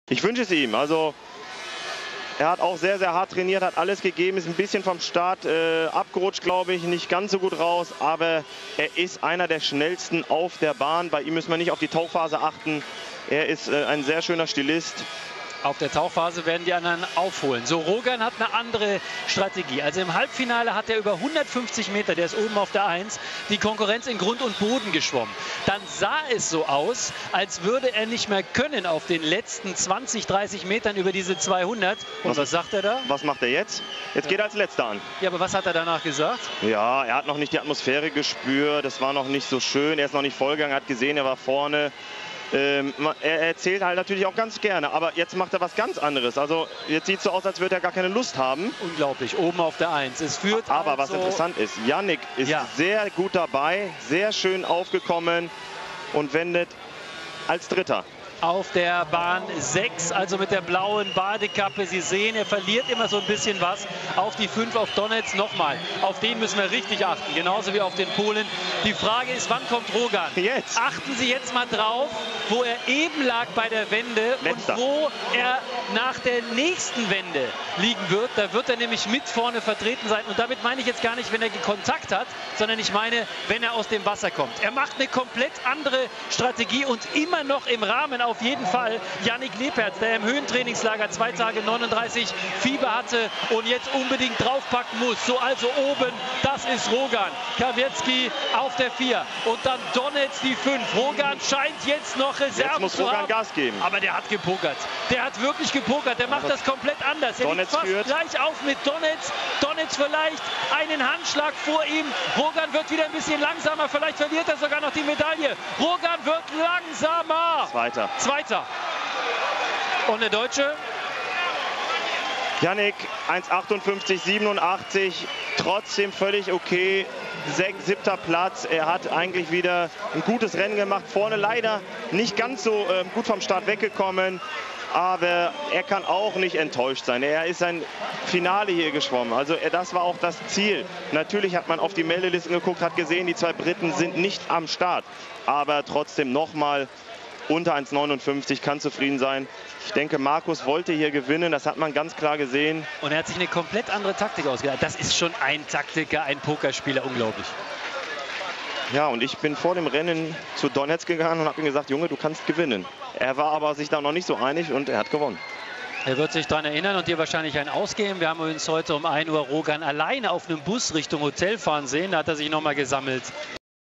Men's 200m backstroke European Swimming Championships 2010 Stanislav Donets
1. Stanislav Donets (Russia)    1:57.18
2. Markus Rogan (Austria)         1:57.31
3. Benjamin Stasilius (France) 1:57.37
4. Radoslaw Kawecki (Poland)
5. Sebastiano Ranfagni (Italy)
6. Nick Driebergen (Netherlands)
7. Yannick Lebherz (Germany)
8. Eric Ress (France)